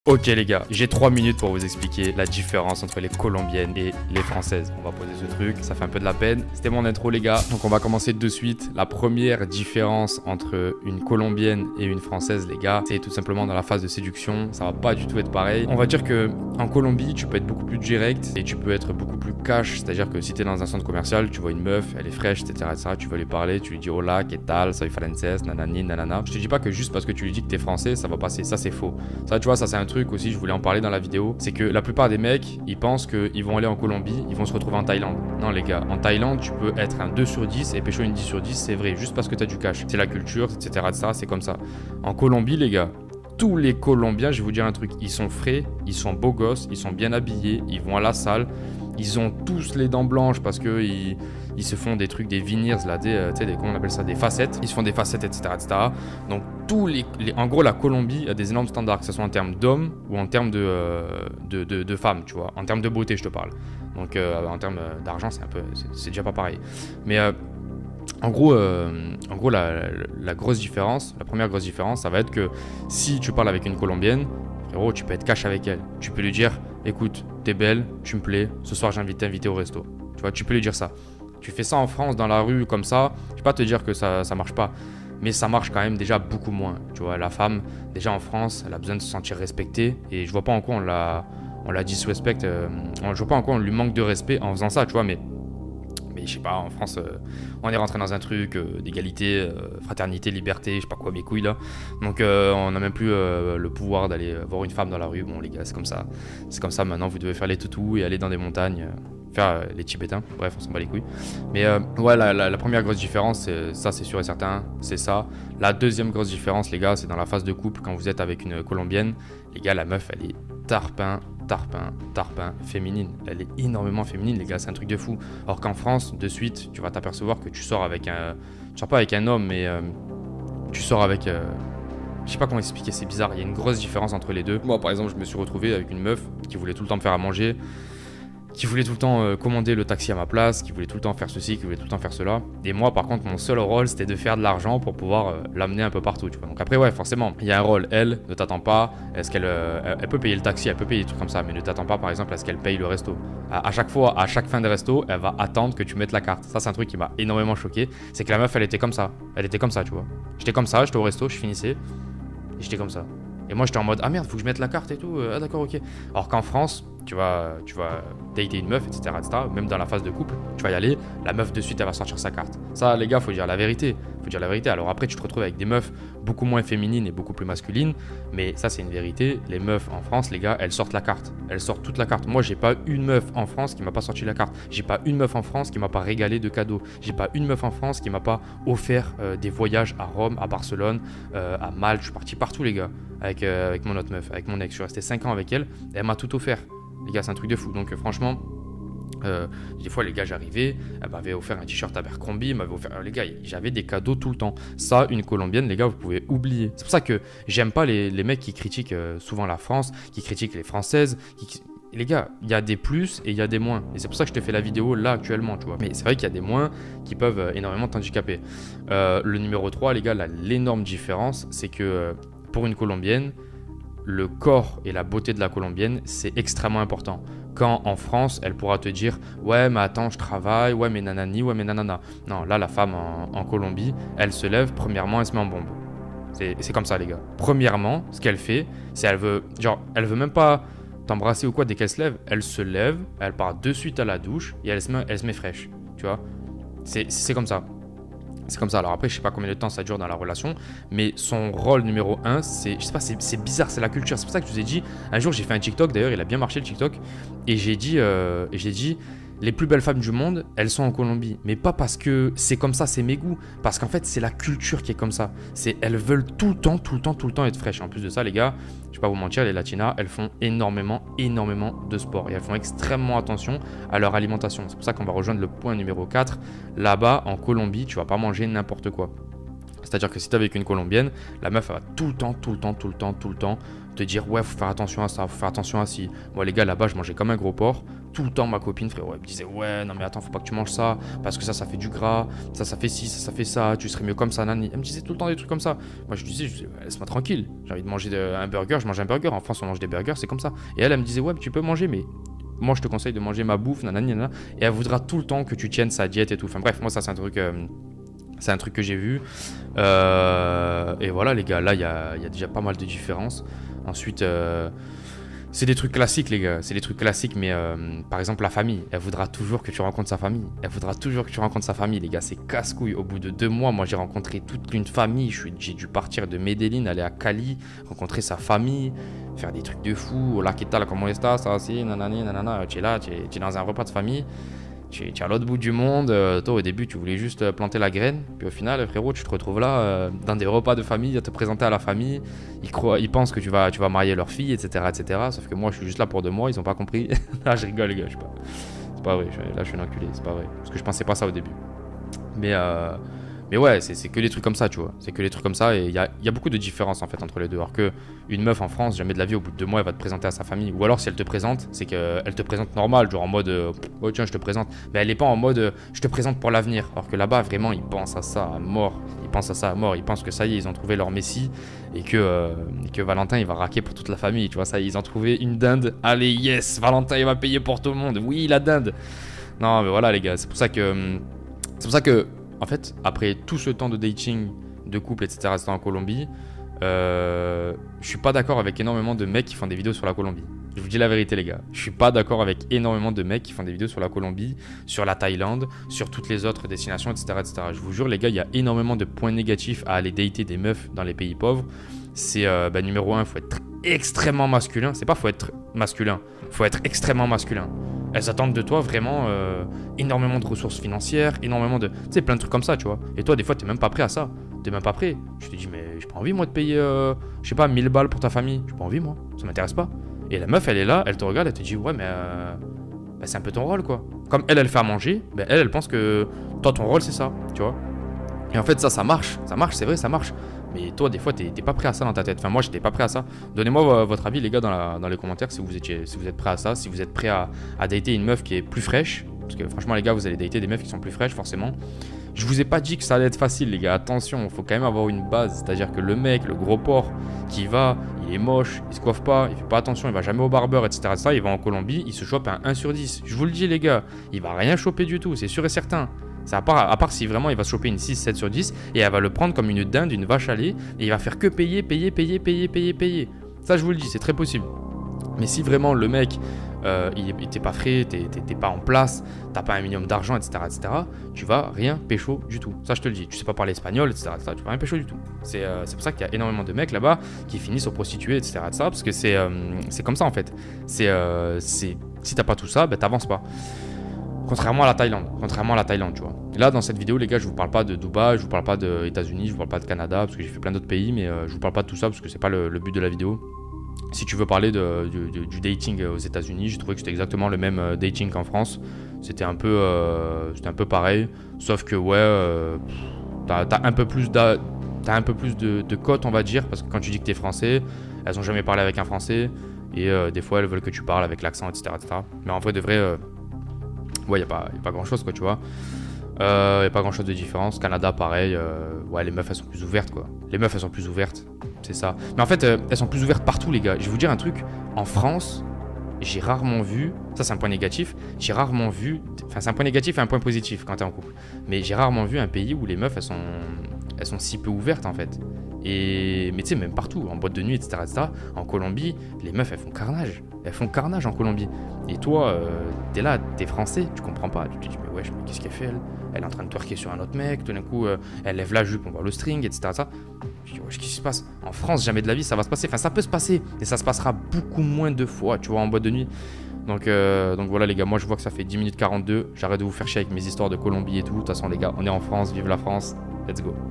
LES DIFFERENCES ENTRE UNE FEMME COLOMBIENNE ET FRANCAISE
🚨 Copie-Colle ma Méthode ! 🚨
➜ Vivre de Youtube en 90 Jours !

Aujourd'hui on voit toutes les différences entre une colombienne & une française !
Les marchés sont bien différent, et c'est ce dont en parle dans la vidéo !

Séduction, nofap, redpill..